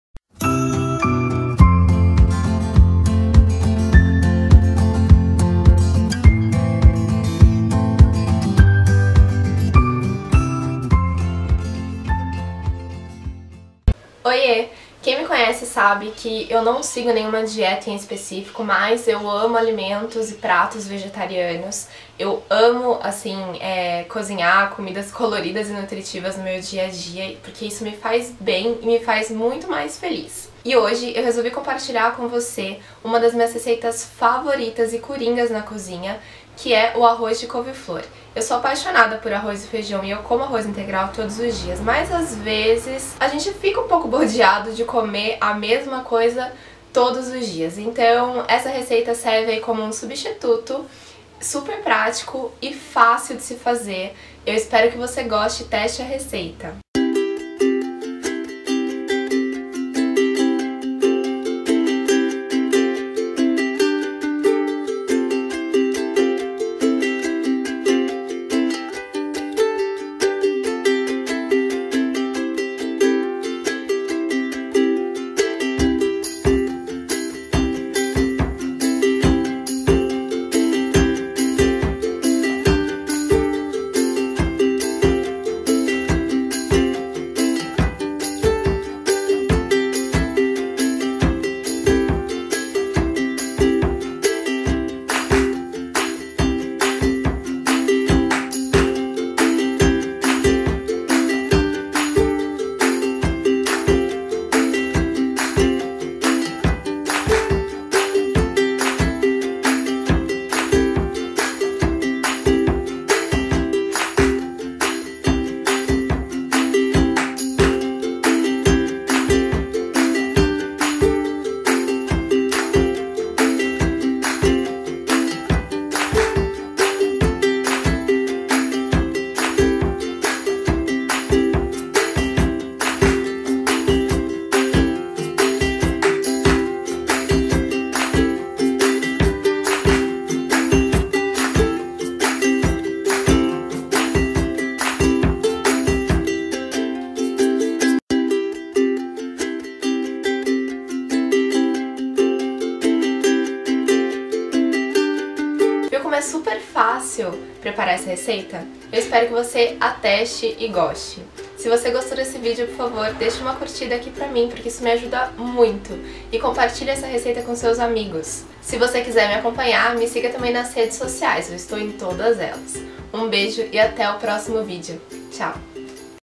Arroz de Couve-Flor (alerta: fitness!) - Por Maiariane Duarte - Blog Le Papillon
Para sair da rotina, hoje eu compartilho com você uma alternativa para o arroz de cada dia. Além de ser uma delicia, o arroz de couve-flor ainda é saudável, fit e super fácil de se preparar. Ele é o acompanhamento perfeito para as suas refeições.

Curta a page no Facebook: @maiarianeduarte